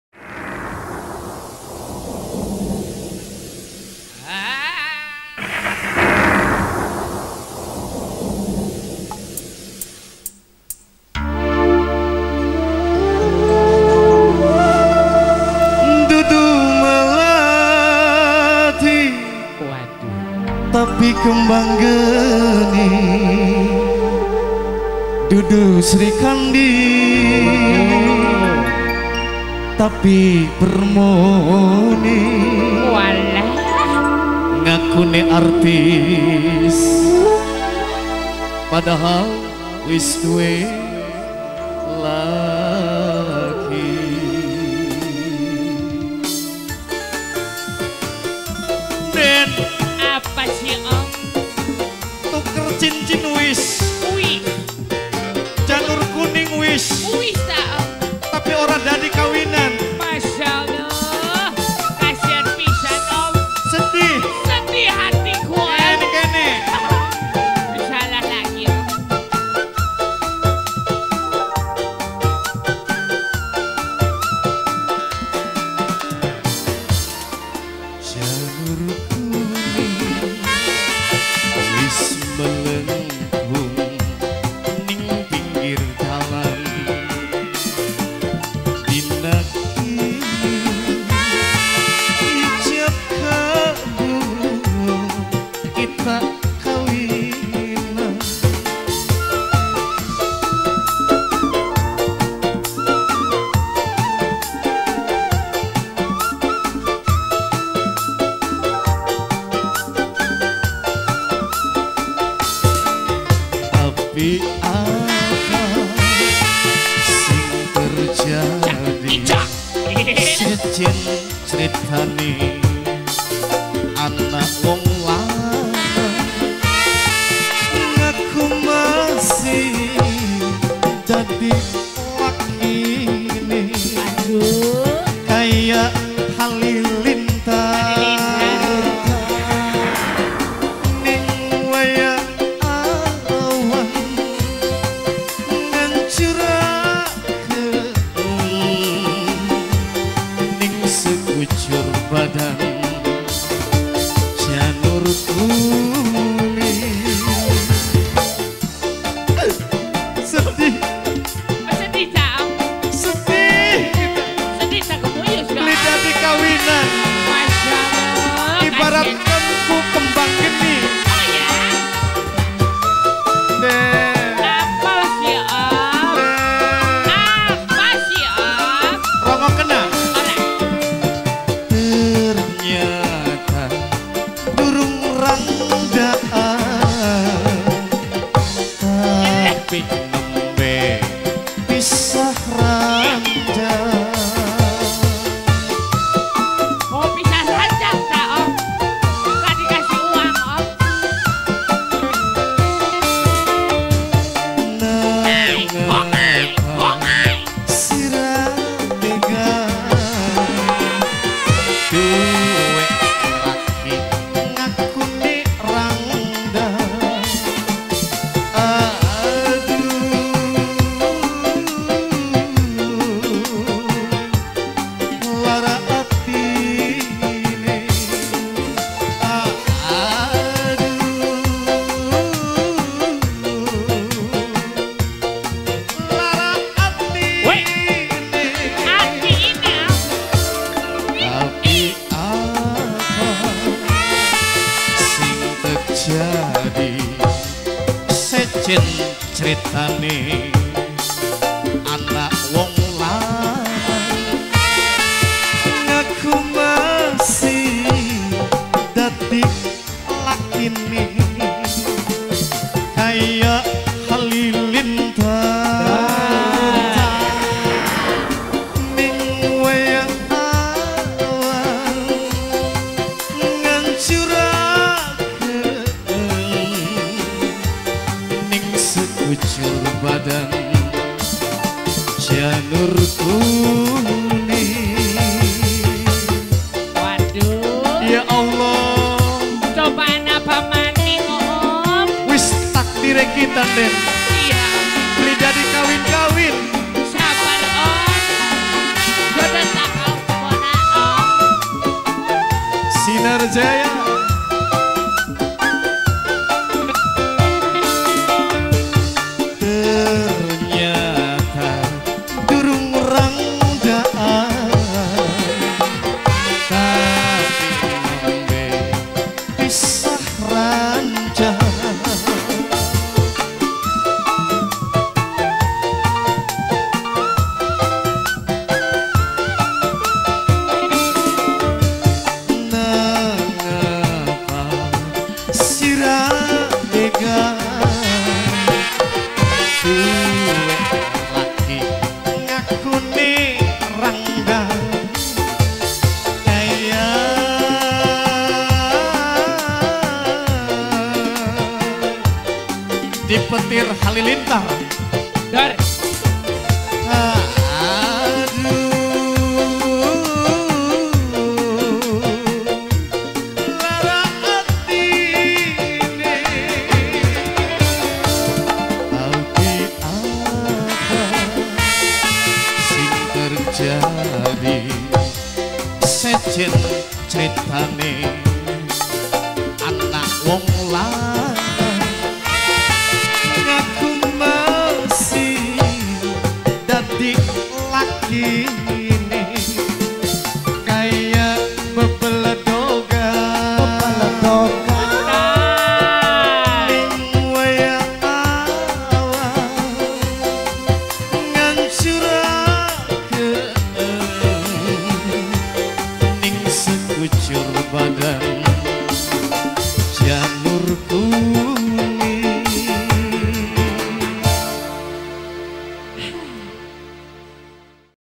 2 SINAR JAYA @ Cupang Gempol Cirebon | Khitanan Ari Alfiano
Hub. Facebook Cupang,22 November 2018
Khitanan Ari Alfiano

Tempat:
Blok Cantilan Rt.14 Rw.05
Desa Cupang Kec.Gempol - Cirebon

Keluarga Hajat:
Kel.Bpk.Suwano & Ibu Maryatun